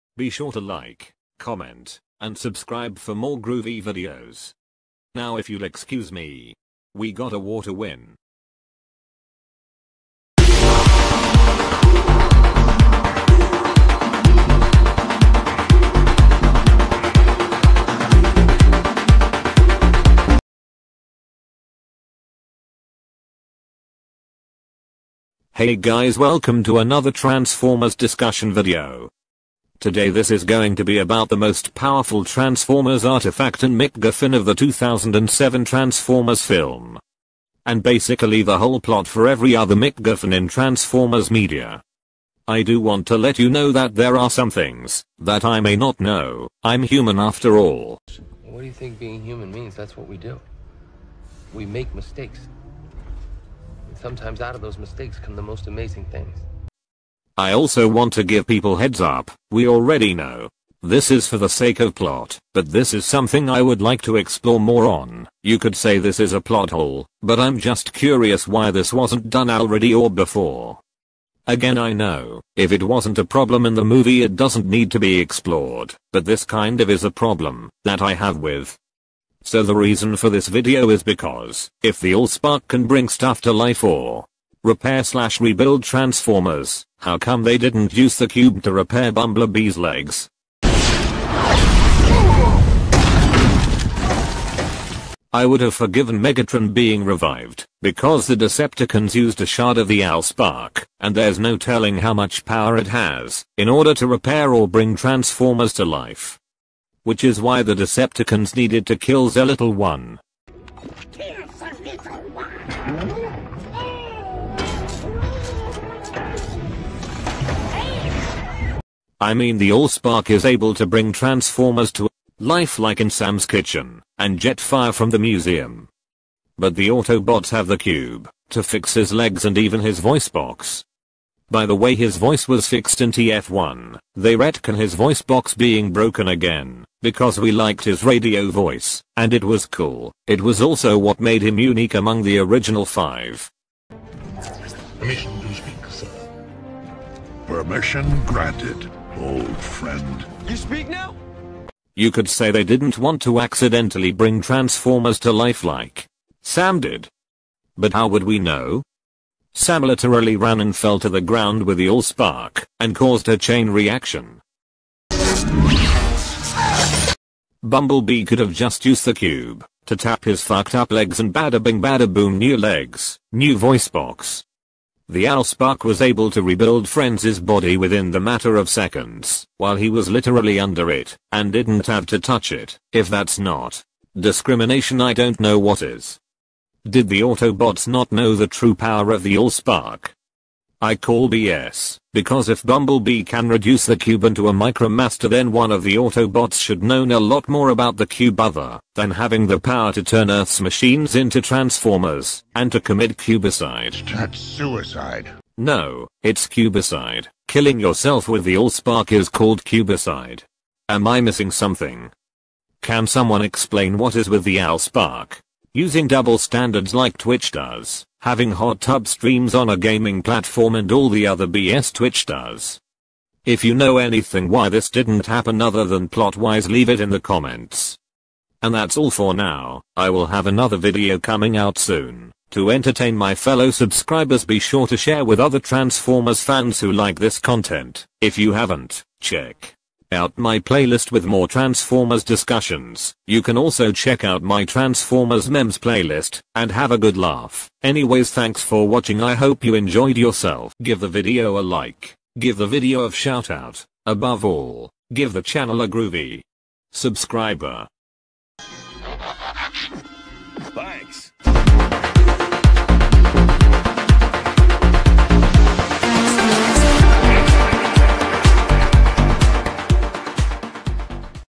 The Allsparks double standard
hey guys I hope you have an awesome 4th of July. This video is going to be about the Allspark being able do things for the Decepticons and not for the Autobots, it maybe a silly video to make but I like to explore more on this subject and see if anyone would understand where I'm coming from also get a logical answer behind this movie.